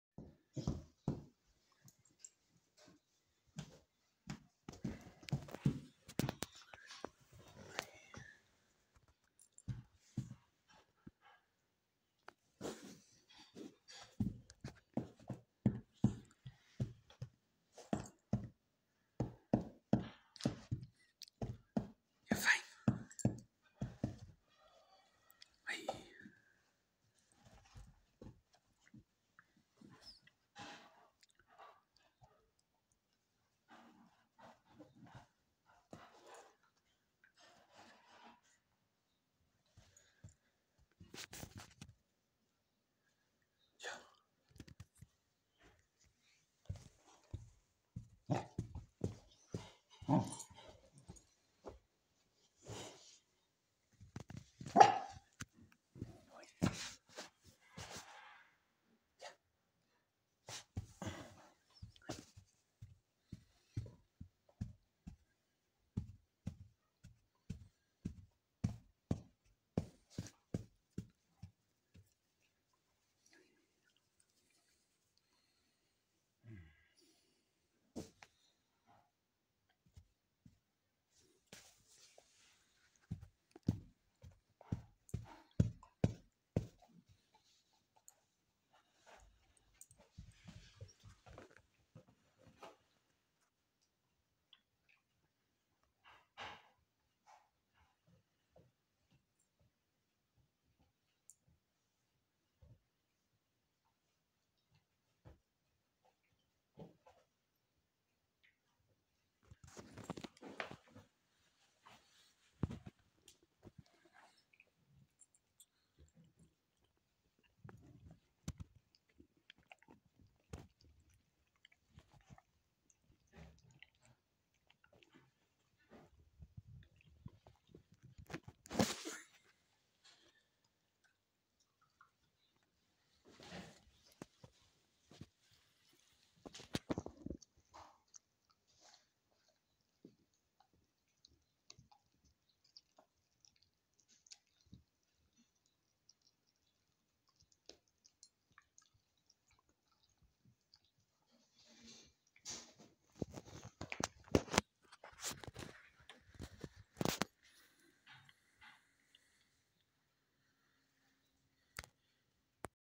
Polly 'Cohetes-Dachshund' come un regalo y baila con su trasero
'Raketen-Dackel' frisst ein Leckerchen und zeigt uns dabei mal wieder ihren Hintern